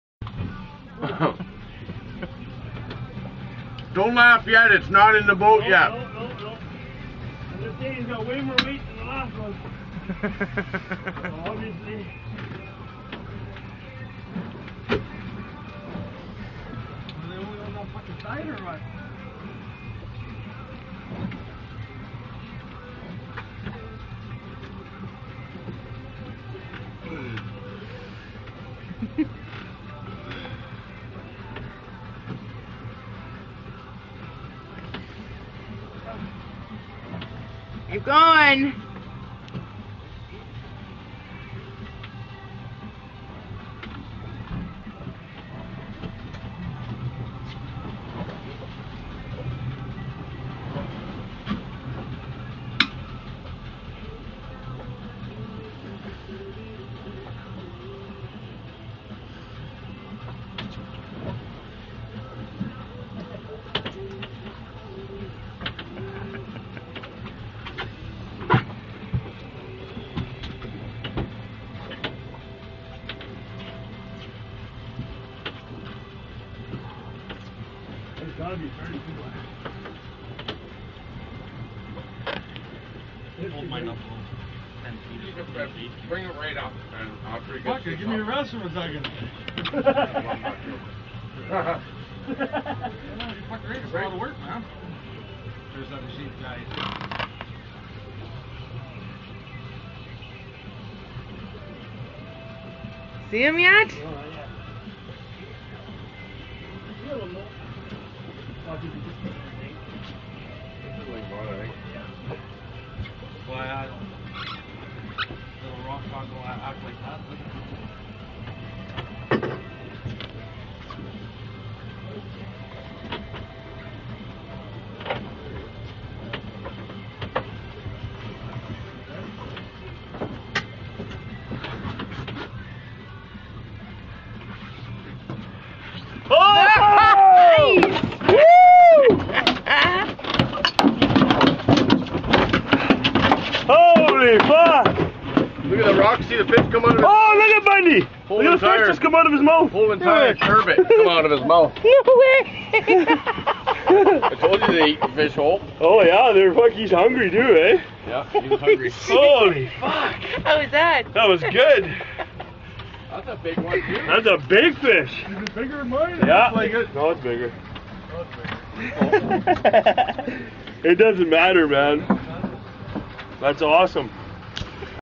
Rupert Ling Cod
Swanny reels in a 35 lb Ling and is a bit excited